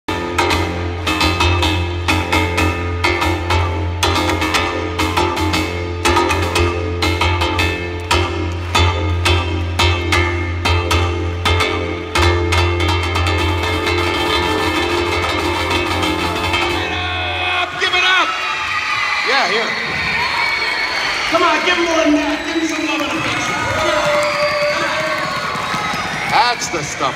ROB THE DRUMMER pounds the WAVE DRUM with a young STAR!
Rob Gottfried highlights many amazing drumming innovations offered by his SPONSORS!  The WAVE DRUM ~ by KORG ~ gives Kids a great venue for building SELF-Esteem!  Rock on Kids!!  For more info on Rob's Self-Esteem concert go